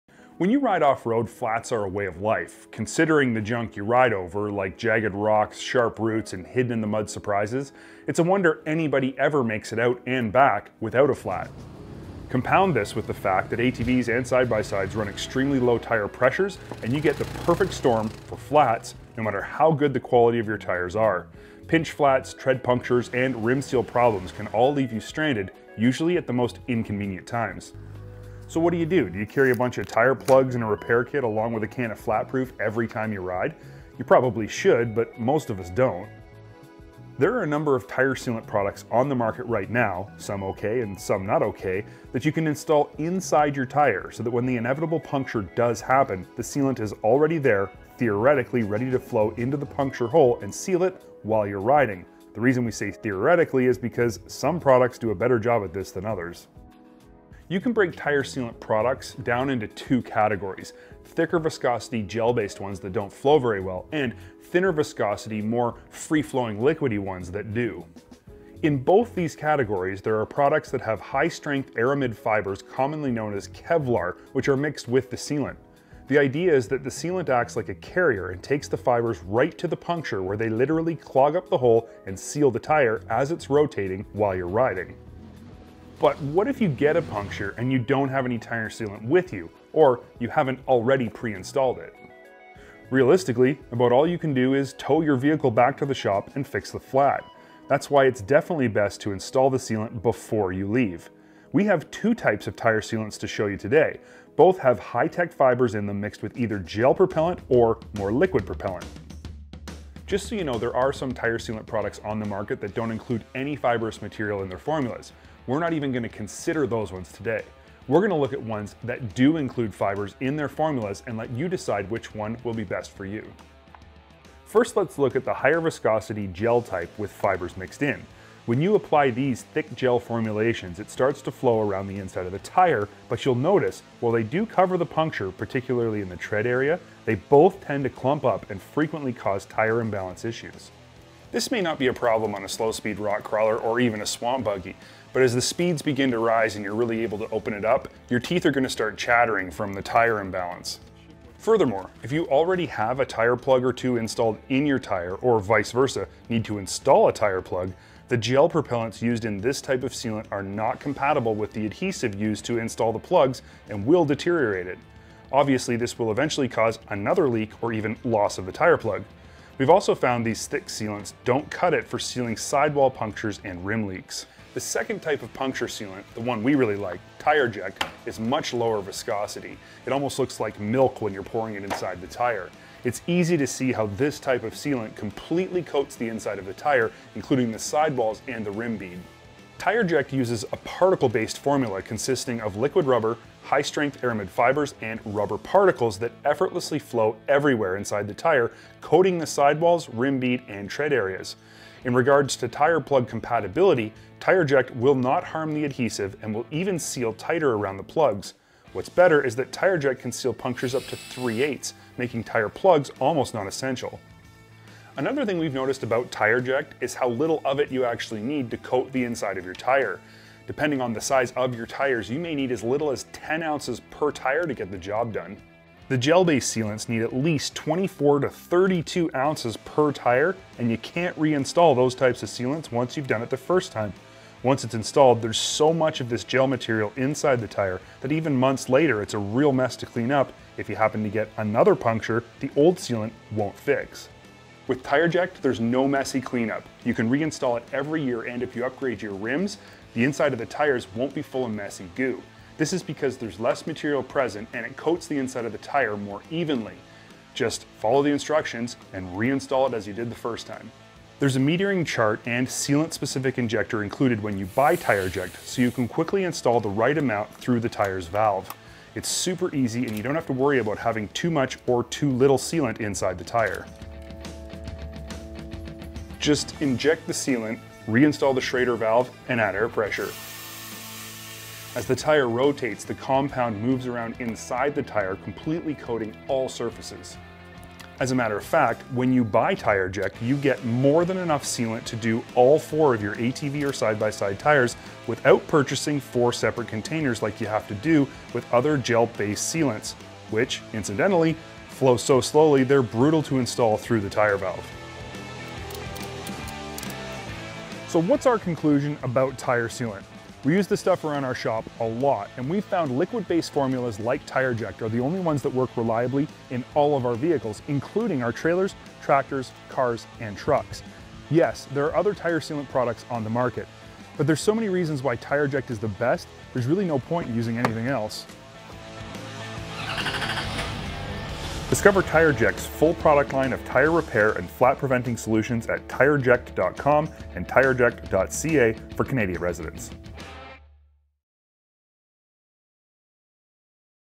TireJect - The Best Tire Sealant To Repair and Prevent Flat Tires
Luke compares the effectiveness of TireJect's particle-based liquid rubber formula consisting of high strength Aramid Fibers to two of the most popular Gel-based tire sealants on the market. See how TireJect Tire Sealant requires a fraction of sealant volume while maintaining the upper hand in protection.  TireJect effortlessly flows everywhere inside the tire, coating sidewalls, rim bead, and tread areas to instantly seal and protect your tires from punctures up to 3/8-inch!

to purchase from Kimpex